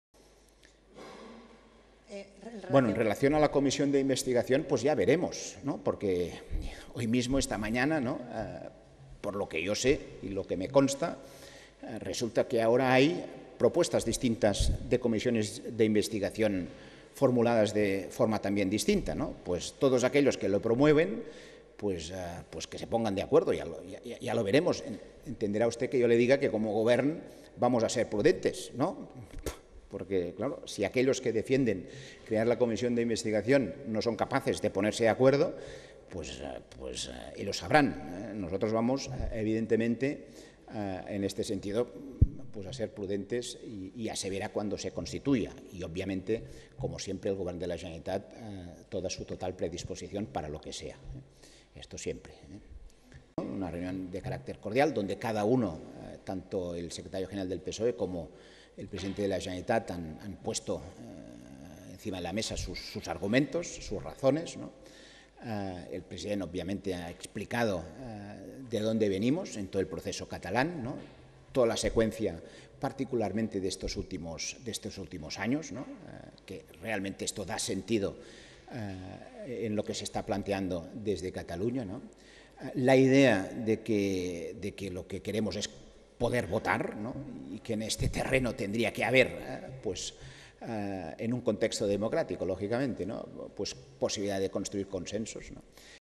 Govern no ve clara la comisión de investigación a Pujol
El portavoz de la Generalitat, Francesc Homs, ha explicado este miércoles que el Govern no tiene "la certeza" de que habrá una comisión de investigación en el Parlament sobre el patrimonio oculto que ha confesado el expresidente Jordi Pujol.